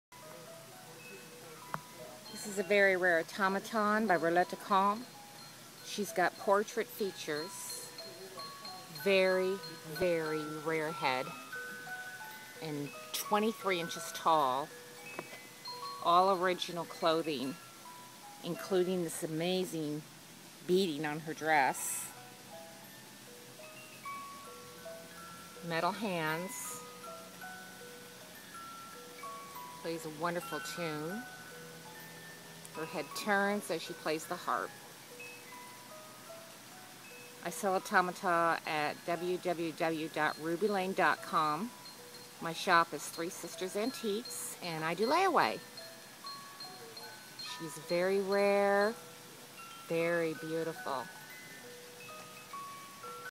Rare 23" automaton harpist
Rare portrait head
Original gown
Available for sale on layaway